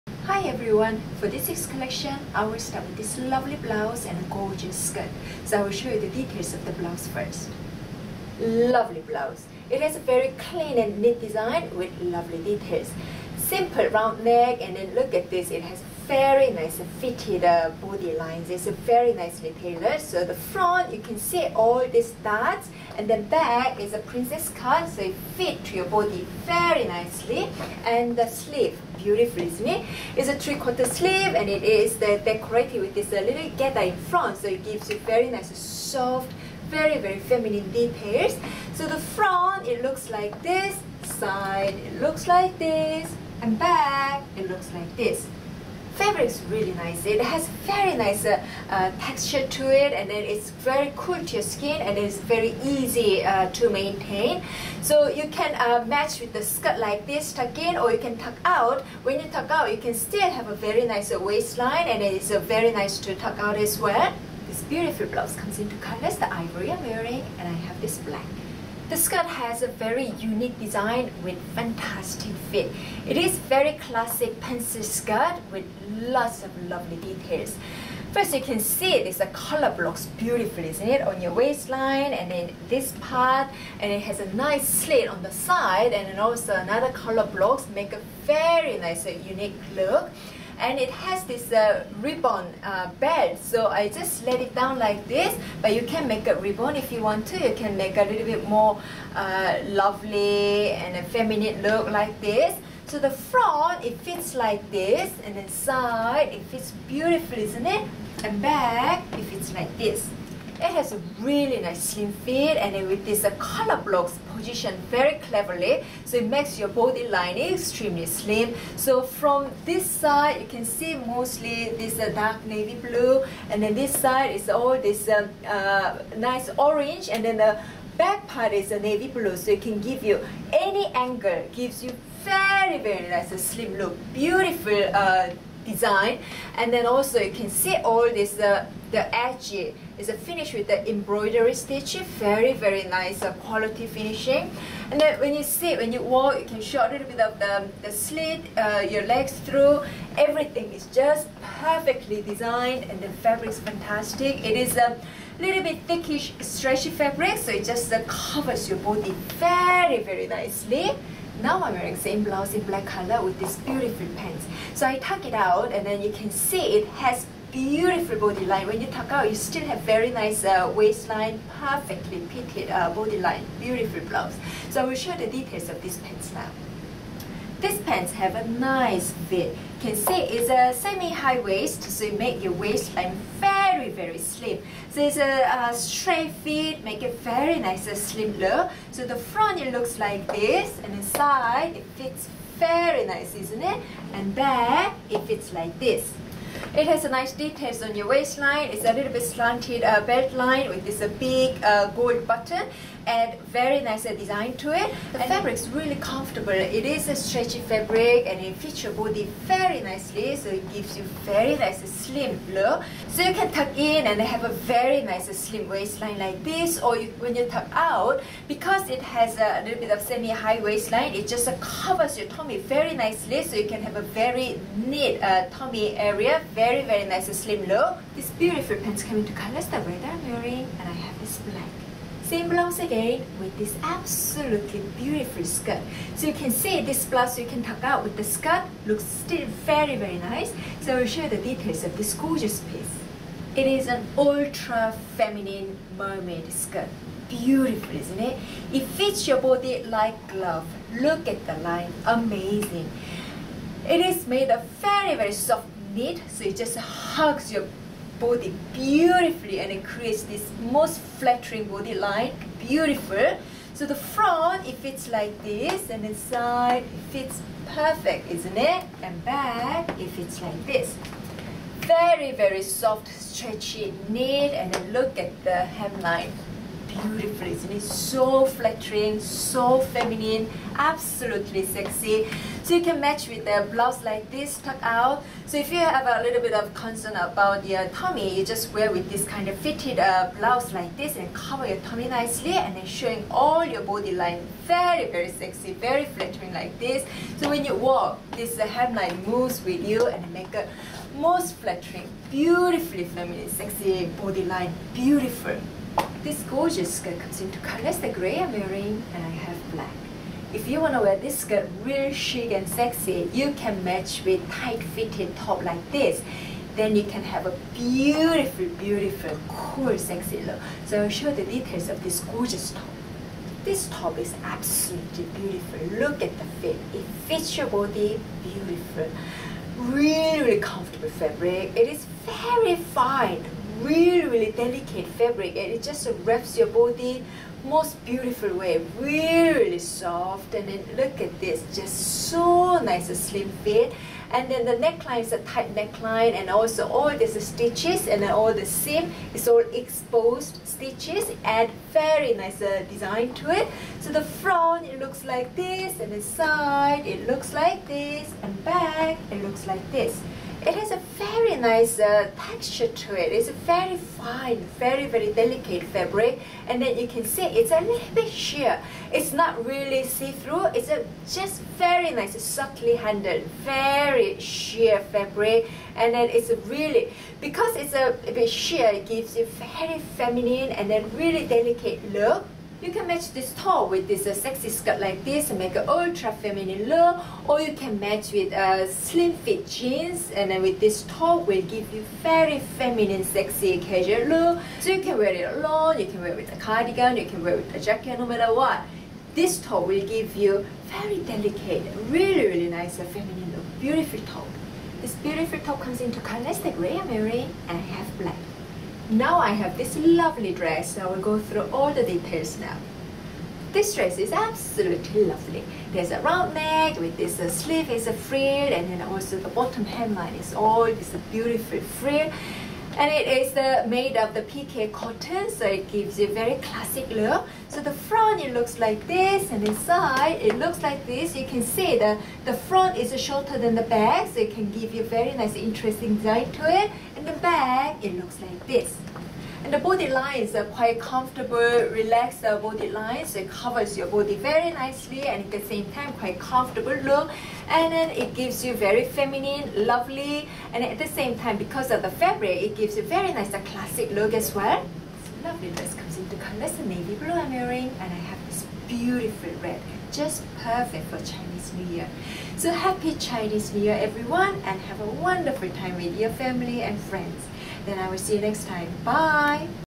What to wear this week-12 February 2018
Christine shares styling ideas with this week's new arrivals. Visit thestyleseoul.com for more fashion styling tips.
*Audio problem is fixed.